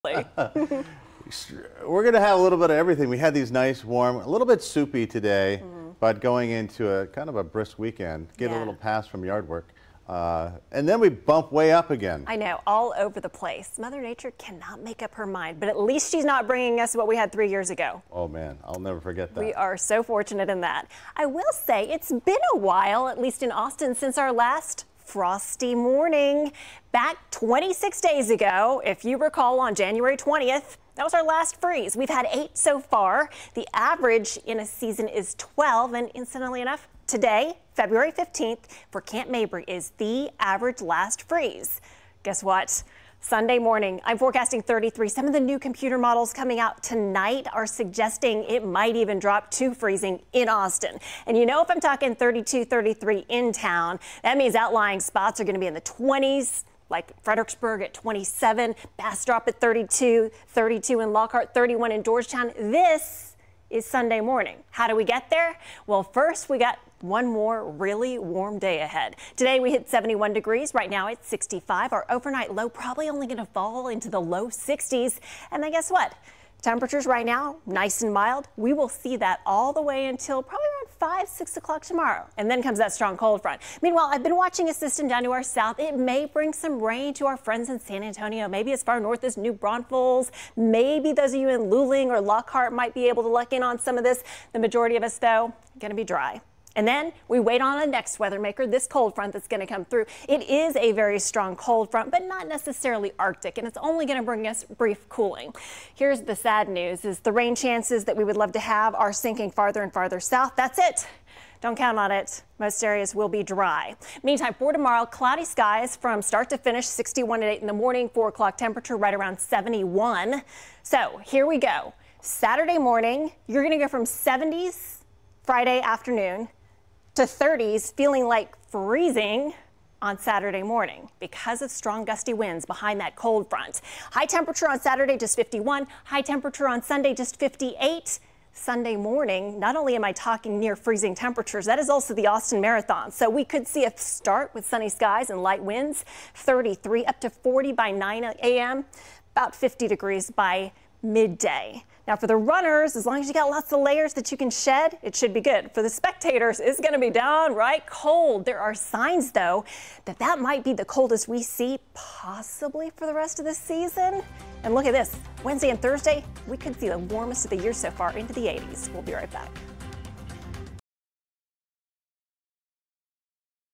From 70s to 30s! Strong cold front, possible freeze. ChiksClips - Thursday, February 15th, 2024
From 70s to 30s! Strong Friday evening cold front, freeze possible Sunday morning. ChiksClips - Thursday, February 15th, 2024